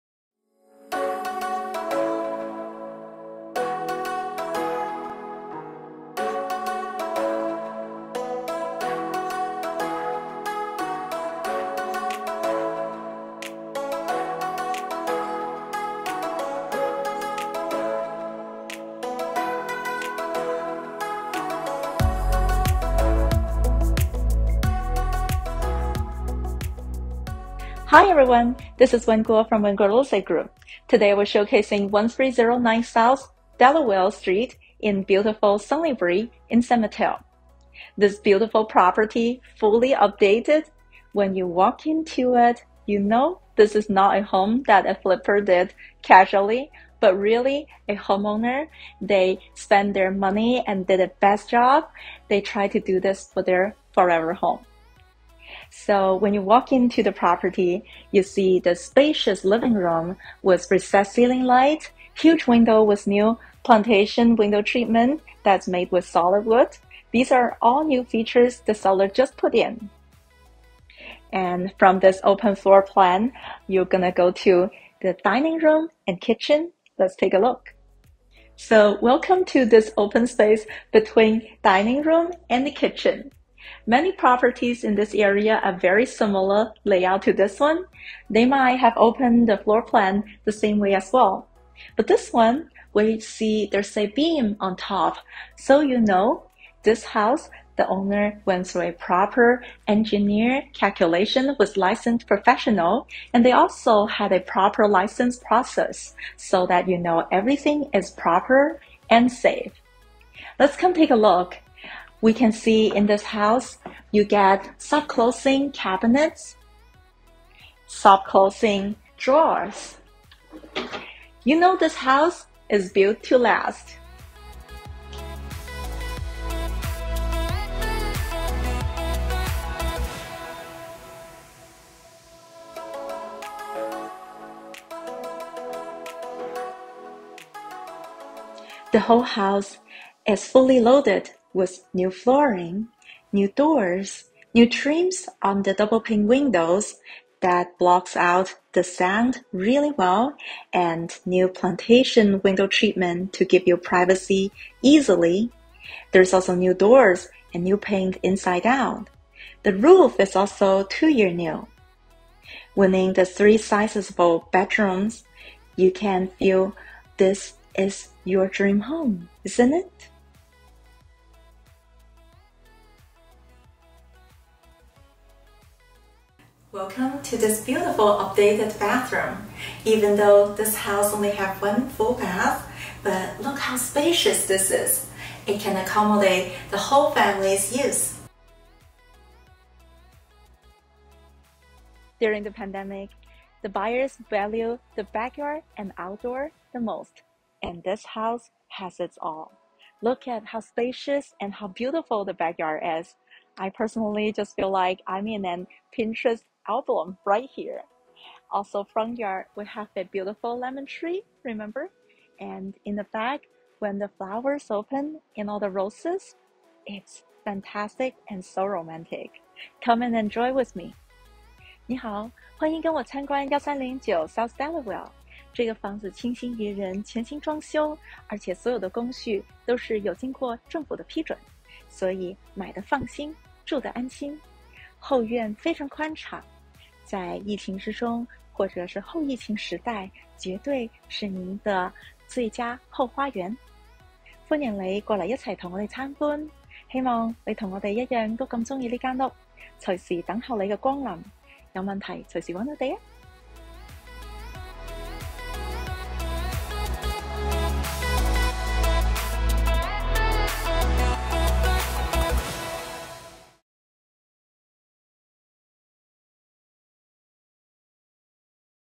Wen Guo Presents | 1309 S Delaware St, San Mateo
Discover Sunnybrae at its finest. An inviting front porch & landscapes lined with a lemon tree & lush shrubs welcomes you. Generous open floor plan for living, dining & kitchen. Large dual pane windows w/ wood plantation shutters let in abundant natural light. Dine-in kitchen is new & opened up, features a large peninsula, trendy color & materials, STT appliances, recessed lights, a perfect spot for warm meals & memorable nights! 3 sizable bedrooms share the updated bath w/ new plumbing, double vanities & shower over tub. New flooring, new doors, new trims on window & doors, new paint inside-out, 2yrs new roof...this home is fully loaded! Private cozy backyard w/ lush new grass & rose garden w/ smart moisture-based irrigation system, paved seating & BBQ area. This home presents $150-200+/- k value on upgrades mostly done with permits! Conveniently located near DT San Mateo. Close to parks, good schools, dining & commute routes. This home promises to create happy memories for generations to come!